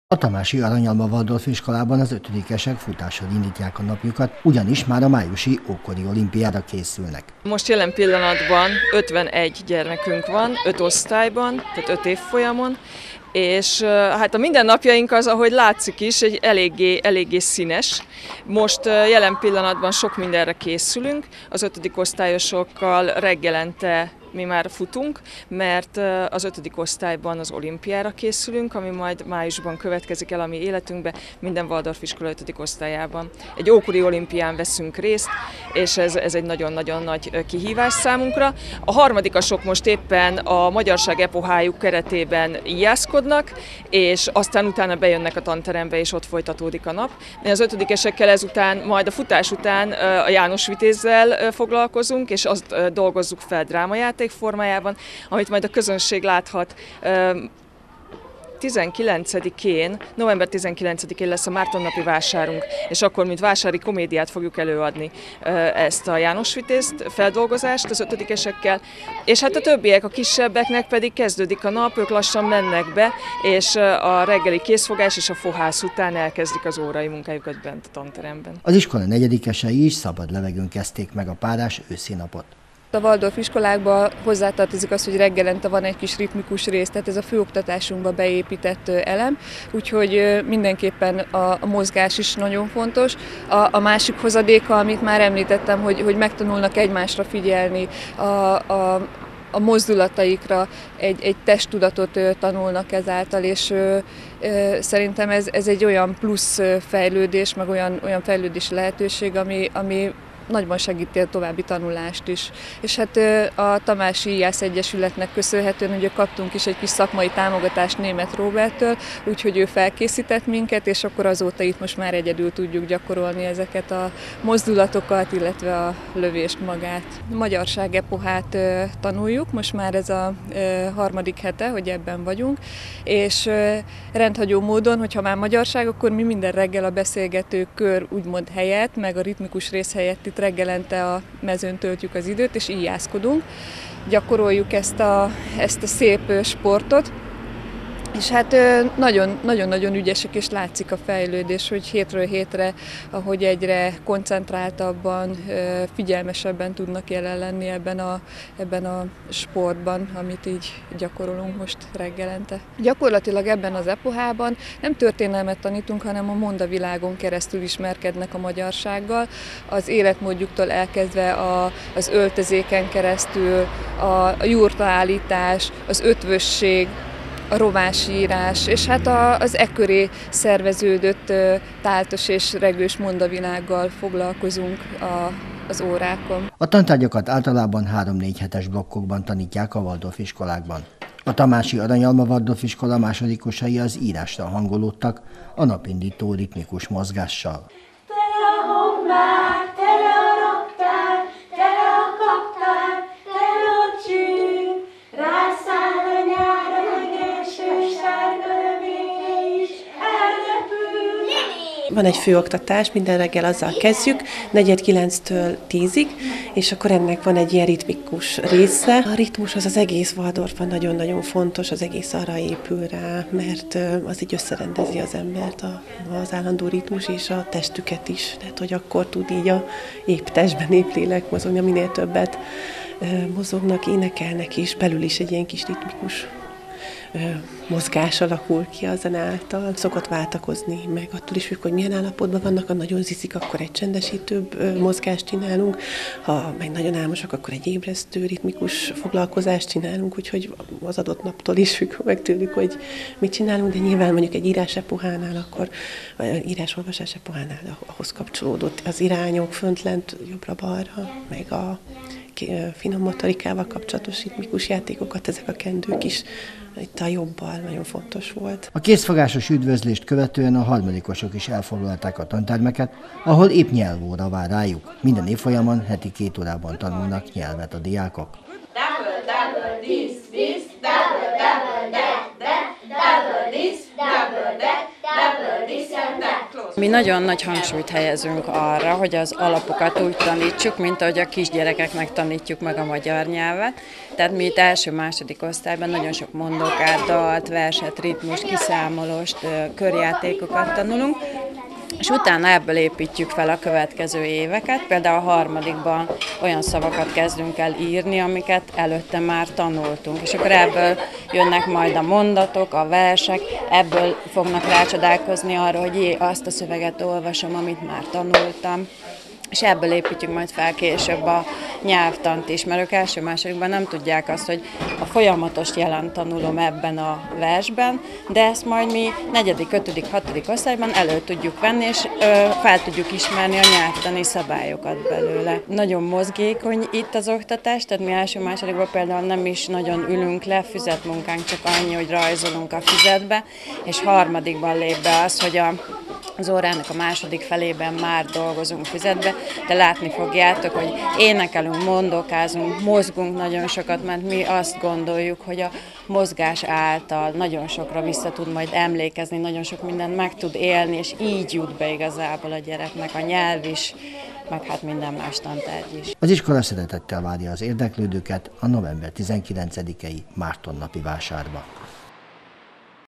Tamási Aranyalma Waldorf Iskola
A Tamási Aranyalma Waldorf Iskolában 5 évfolyamon 51 gyermek oktatása folyik. A Waldorf-pedagógia a tanulás elengedhetetlen részének tartja a ritmust. Az iskolai napok folyamán a koncentráció és ellazulás, a gondolati és gyakorlati tevékenységek, a mozgás és a pihenés, a passzív és aktív részvétel egészséges egyensúlya valósul meg. Minden napnak és minden órának megvan a maga ritmusa. Most pedig jöjjön a kis betekintőnk a waldorf oktatásba.